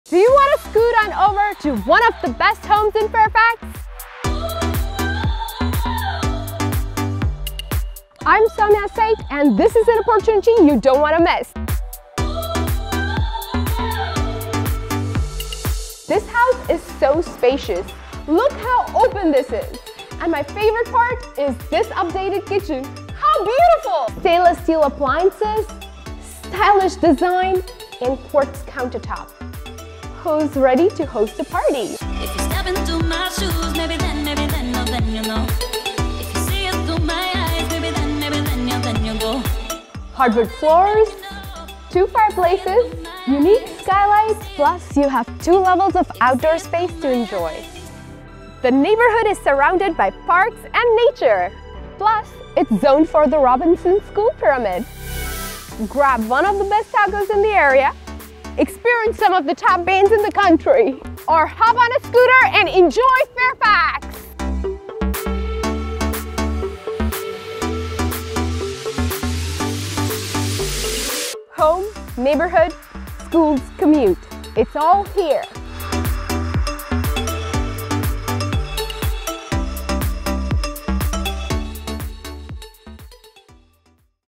Somya Seth's New Listing In Fairfax.
Beautiful townhome in Kingsbury community, zoned for Robinson School Pyramid. Close to George Mason University and Royal Lake.